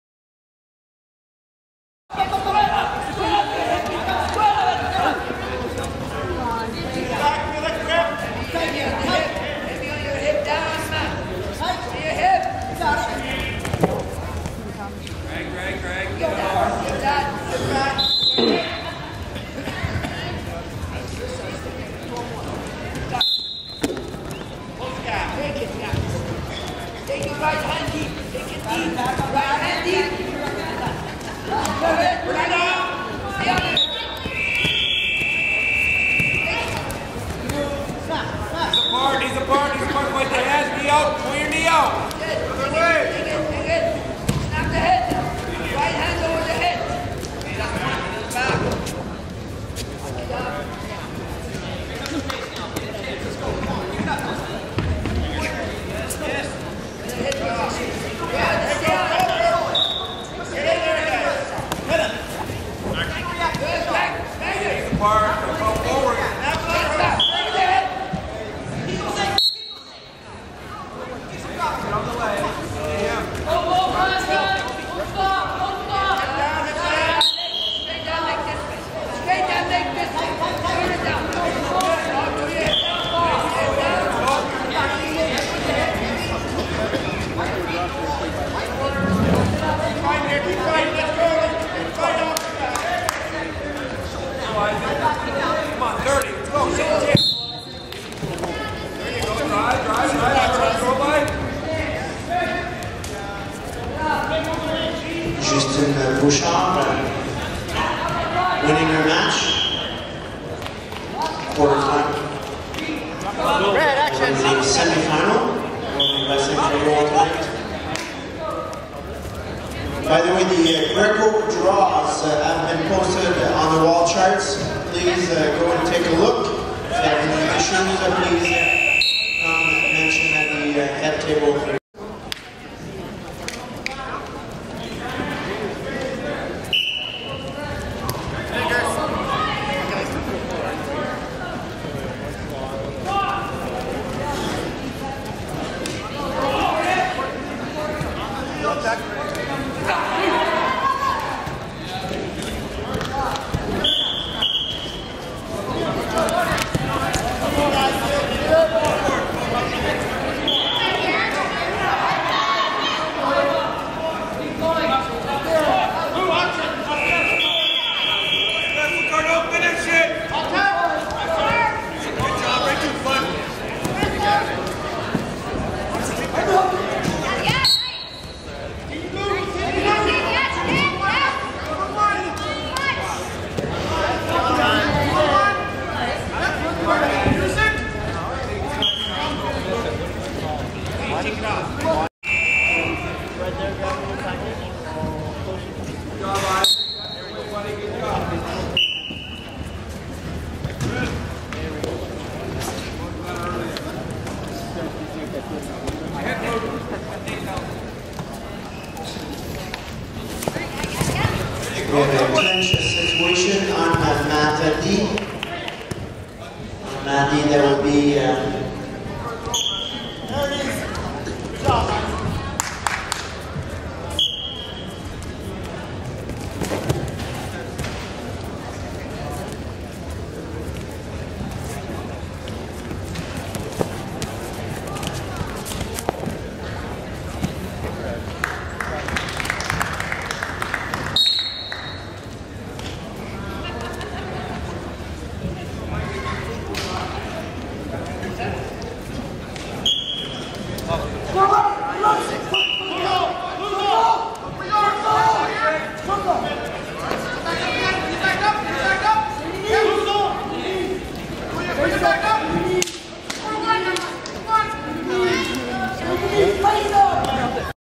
2013 Senior National Championships: 84 kg Tagziev Tamerlan vs. Manvir Sahota
2013 Senior National Championships 84 kg Match Between Tagziev Tamerlan (Team Impact) and Manvir Sahota (Burnaby Mountain Wrestling Club)